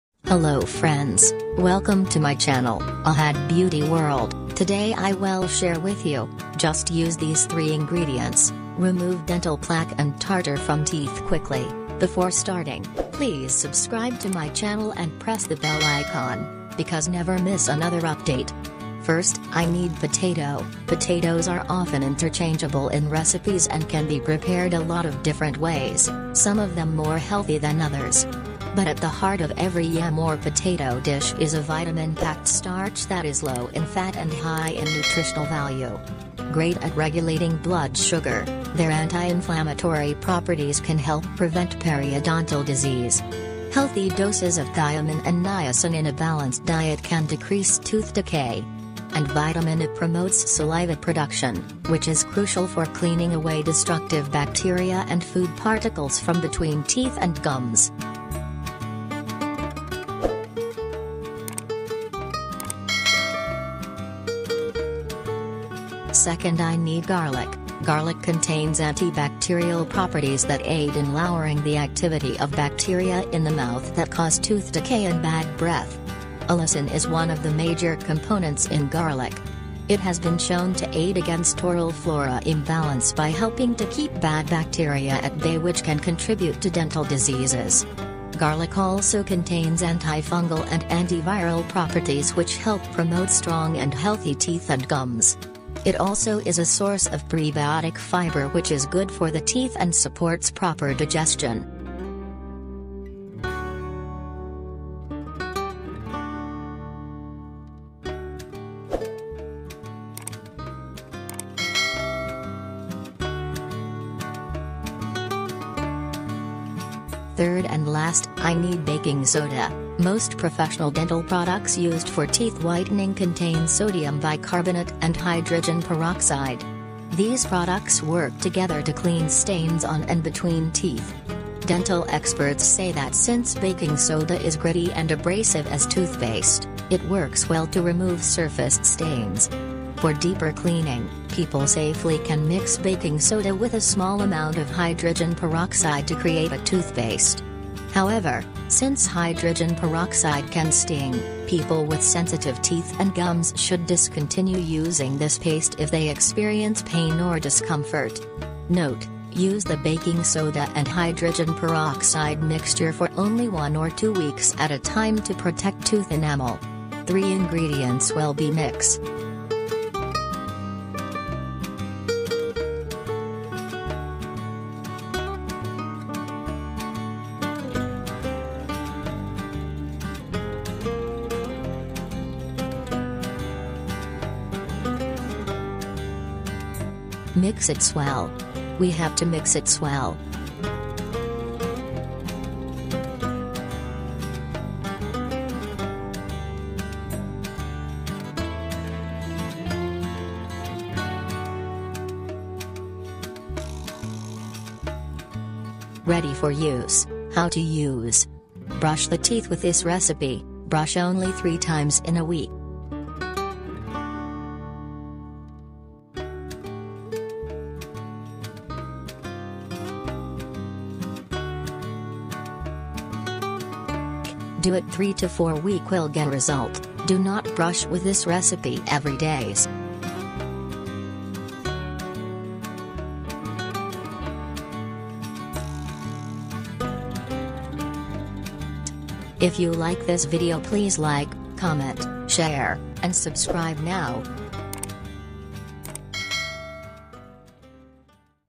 Just Use These 3 Ingredients Remove Dental Plaque And Tartar From Teeth Quickly | Ahad beauty world
Just Use These 3 Ingredients - Remove Dental Plaque And Tartar From Teeth Quickly

this ingredients
-Potato
-Garkic
-baking soda

Brush the teeth with this recipe
- Brush only 3 times in a week
- Do it 3 to 4 week will get result
- Do not brush with this recipe every days.

Film Footage courtesy of Pixabay.com, Used by Permission as a free use. Image(s) used under free license from pixabay.com.